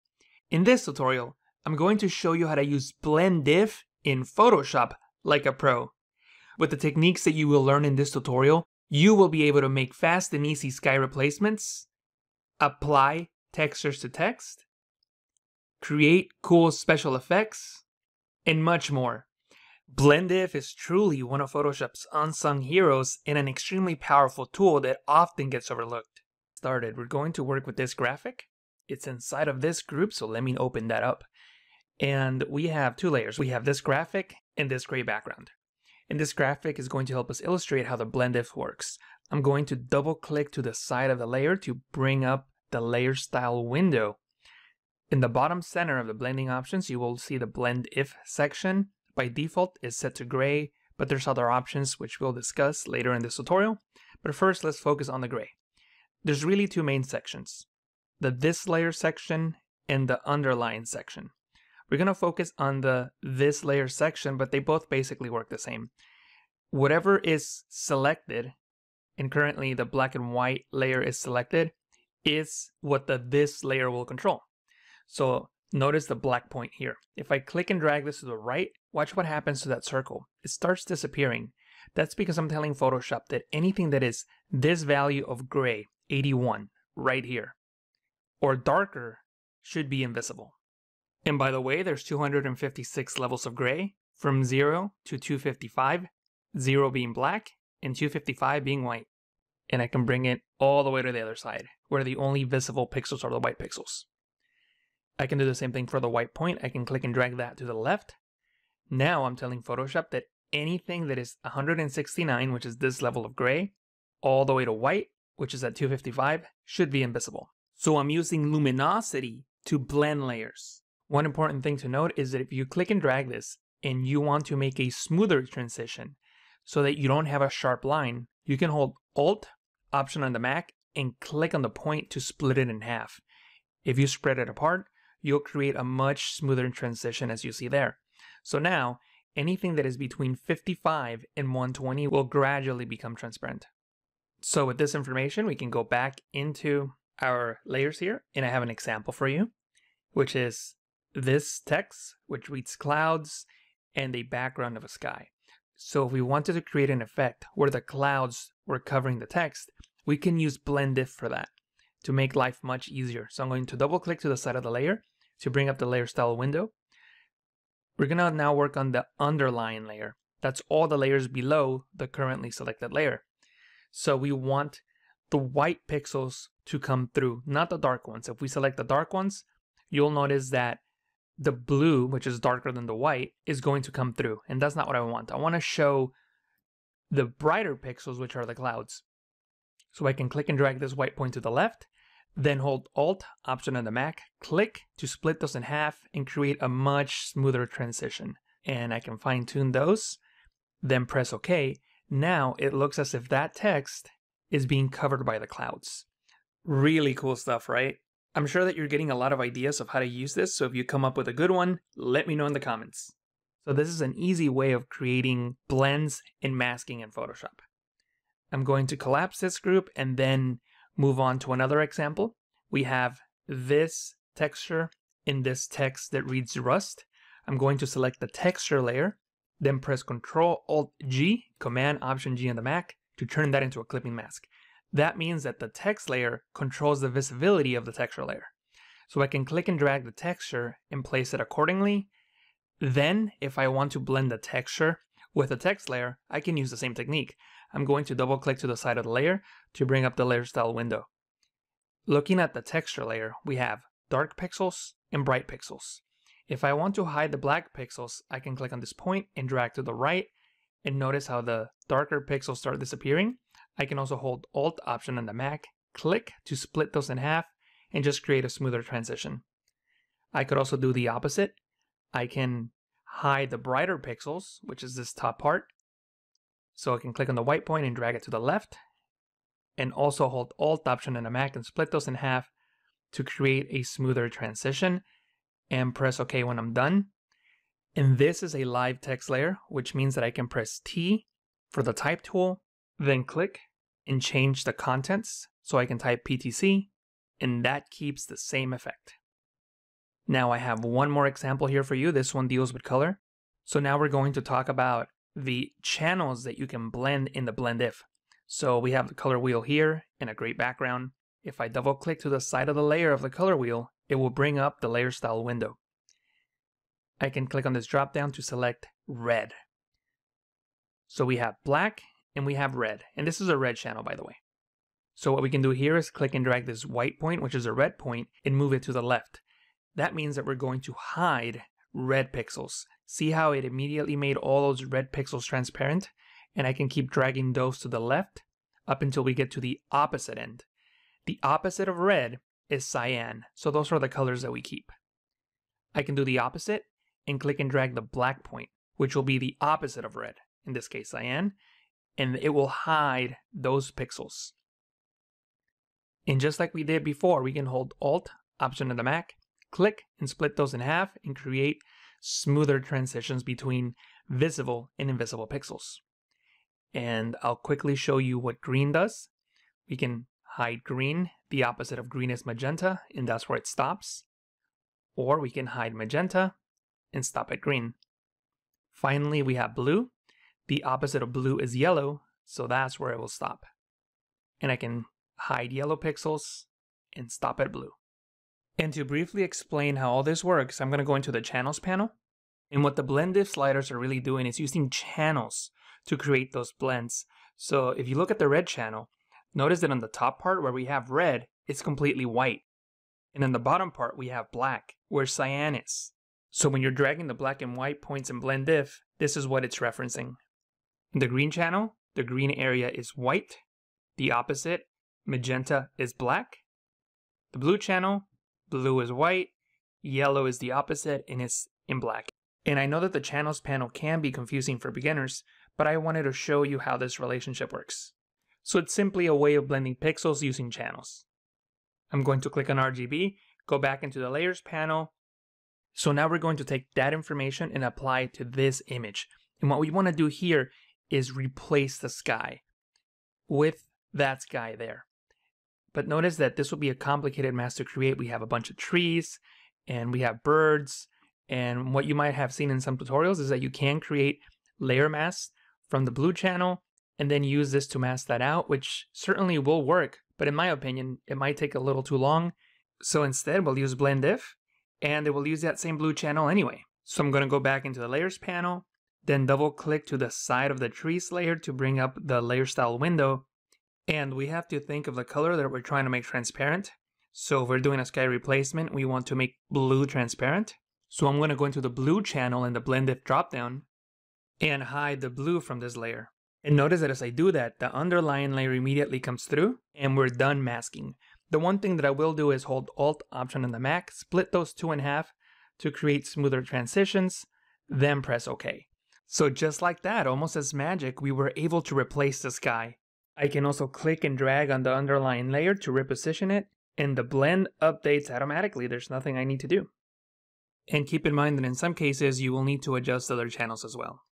How To Use “Blend If” In Photoshop Like a PRO: The Definitive 7-Minute Guide
In this Photoshop tutorial, you will learn how to use "Blend If" like a pro.

This Photoshop tutorial will demystify this scary command to help you create fast and easy blends.

With the techniques that you will learn in this tutorial, you will be able to make fast and easy sky replacements, apply textures to text, create cool special effects, and much more.

"Blend If" is truly one of Photoshop’s unsung heroes and an extremely powerful tool that often gets overlooked.

📘 INDEX - How To Use “Blend If” in Photoshop Like a PRO

04:48 - Blend If Explanation (Gray)
02:54 - Use Blend If To Create Special Effects
04:06 - Apply Textures Using Blend If
05:24 - Blend If Explanation (RGB channels)
07:40 - Sky Replacements Using Blend If

⚡ PTC NEWSLETTER

🏆 PREMIUM TUTORIALS

⚡ FOLLOW ON

⚡ CREDITS